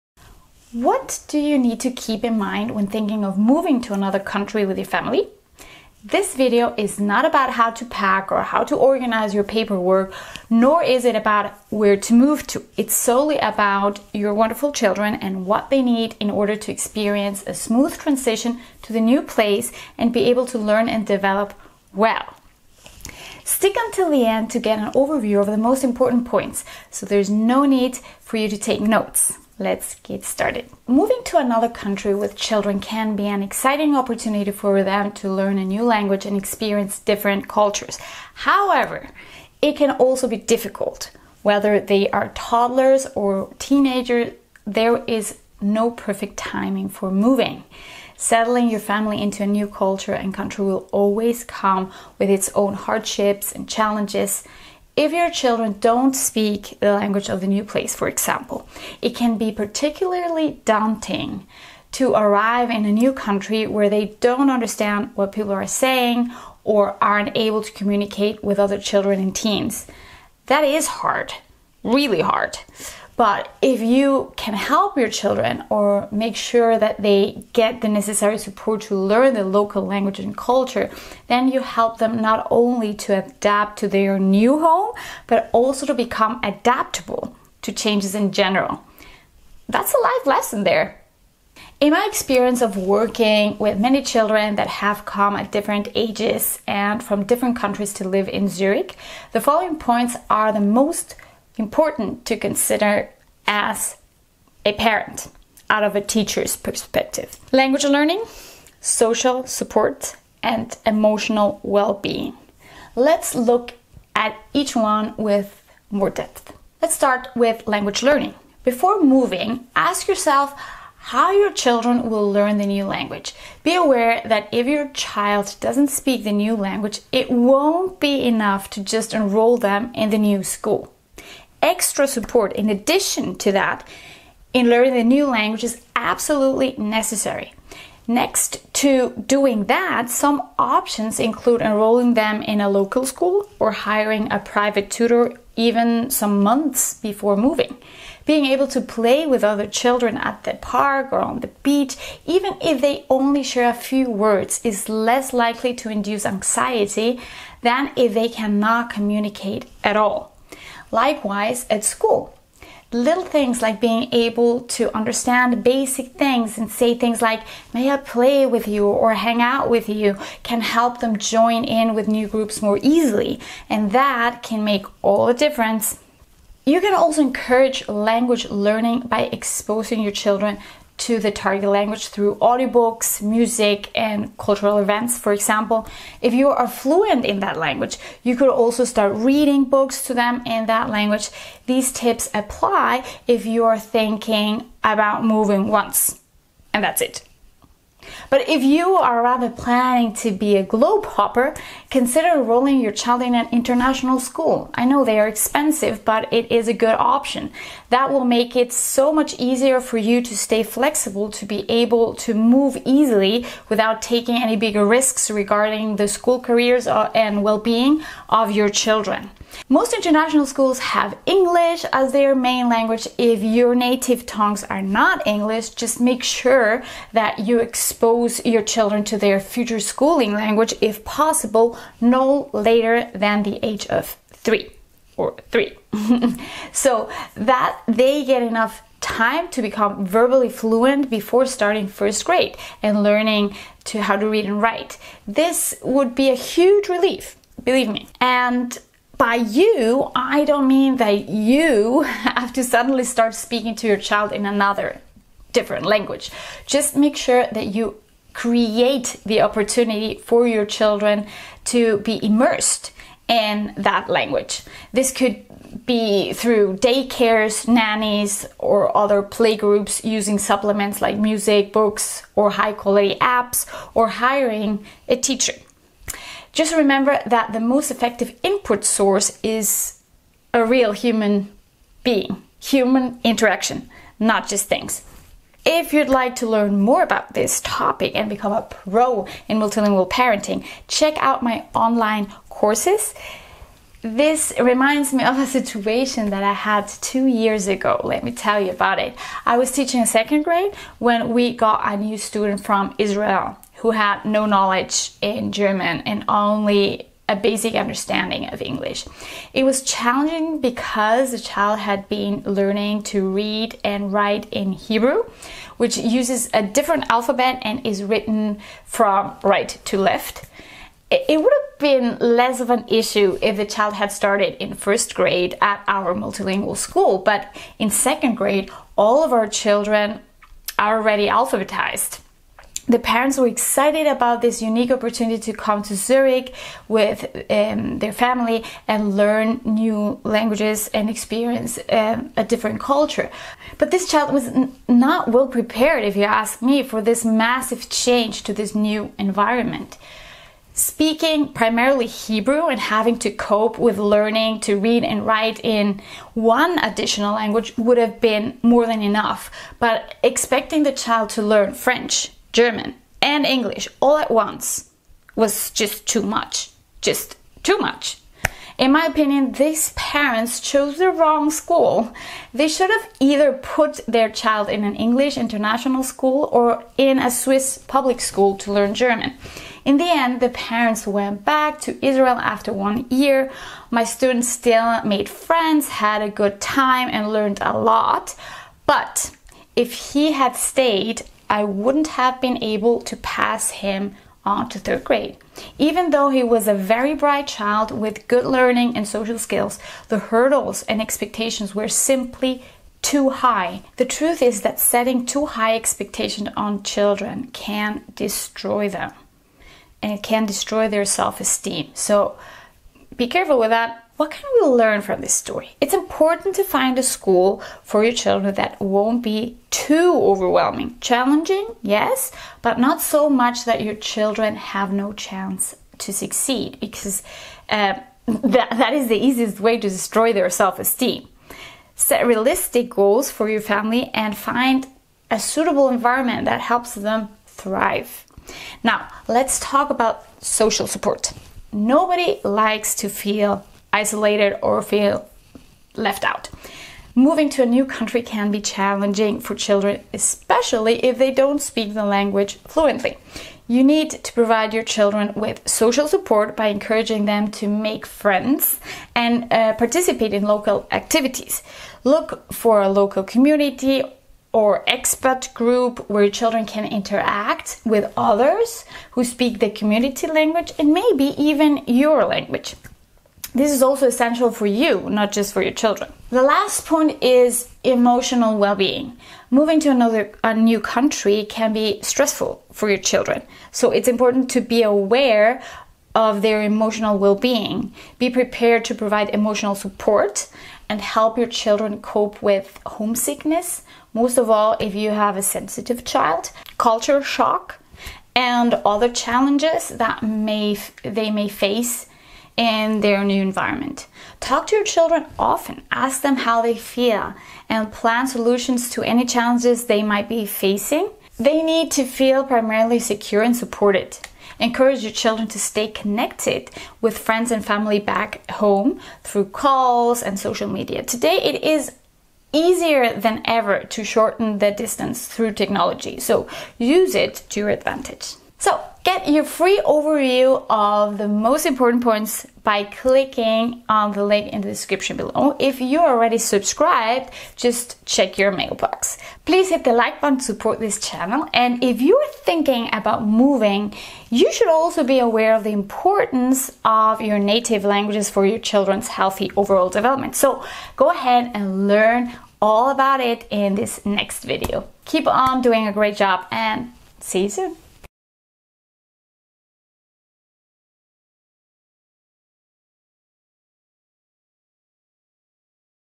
Moving Abroad With Children: What You Need To Think About When Moving With Your Multilingual Kids
Moving Abroad with Children can be an exciting opportunity for them to learn a new language and experience different cultures. However, it's important for expat parents to consider the following factors for their children's language learning and overall well-being.

Download FREE guides with the most important points to consider when raising children in three or more languages:

Time Stamps:

00:00 - Introduction
00:30 - When is the perfect time to move?
01:45 - What kids of expats need
01:57 - Language Learning
07:20 - Social Support
07:58 - Emotional Wellbeing

How to Build a Solid Language Learning Strategy Step by Step

Thank you Paxels.com for the amazing videos and pictures.